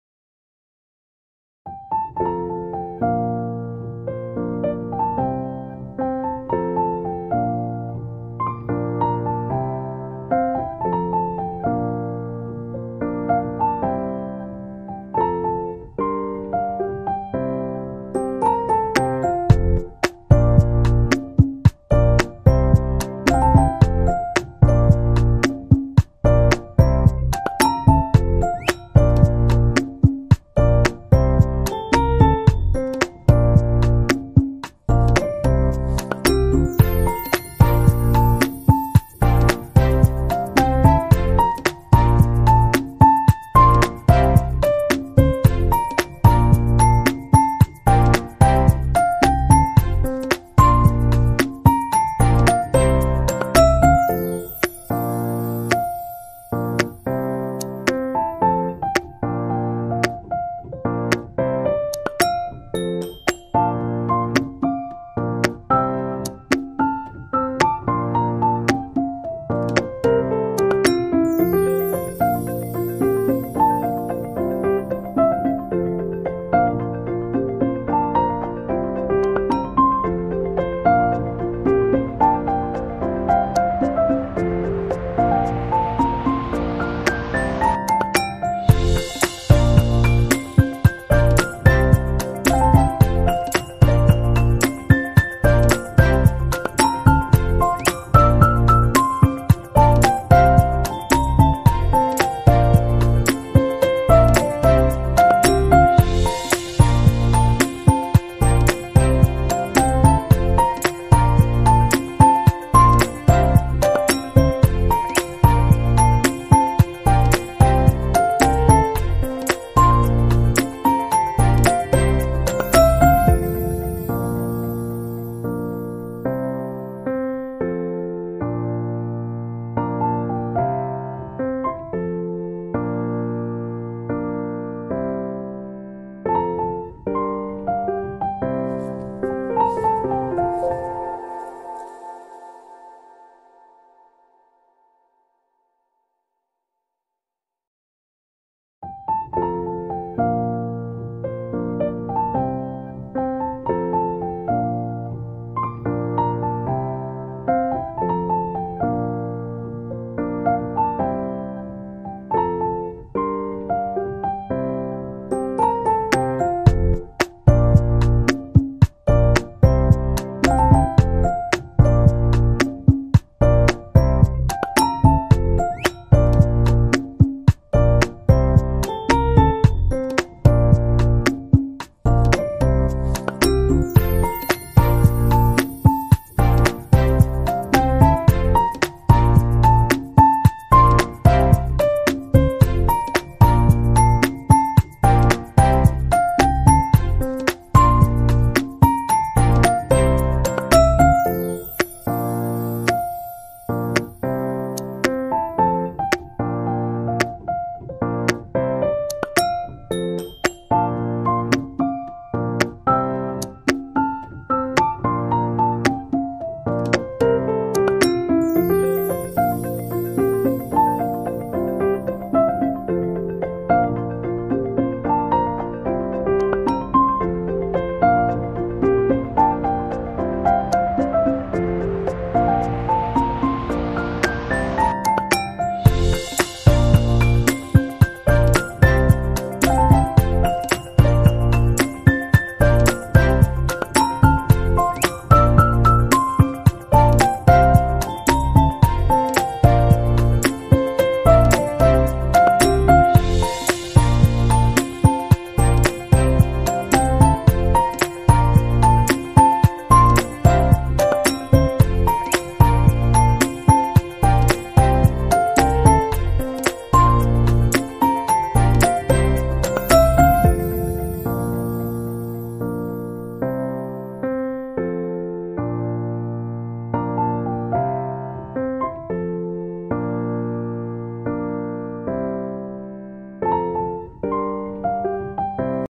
how to make woolen flower amazing woolen craft handmade flowers
hello everyone
here i share simple and easy woolen flower making tricks
you can easily make woolen flower with comb
latest woolen crafts
handmade woolen flower
colourful crafts
simple and easy woolen crafts
handemboridry tricks
because it's totally free
hr creative crafts